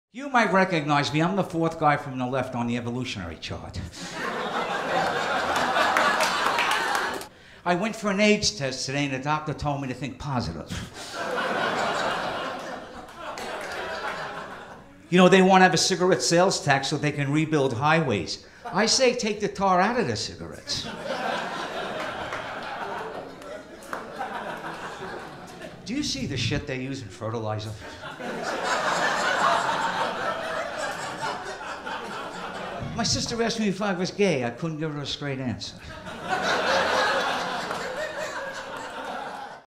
Jay London - Observations (Stand Up Comedy)
Jay has a unique perspective on things.

Come see a live performance if you want to be part of the real fun!